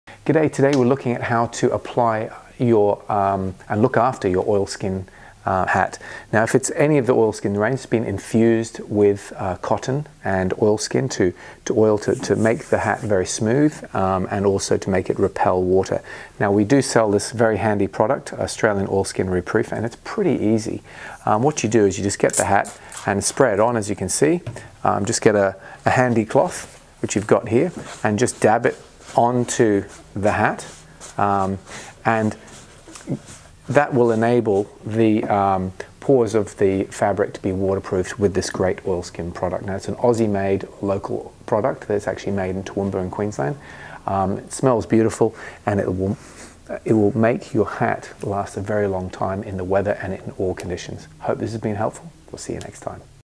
Oil Skin Re Proofing Spray for hats- Hats BY The Hundred
Care for your oil skin hats and coats. Check out this short clip to see how easy it is.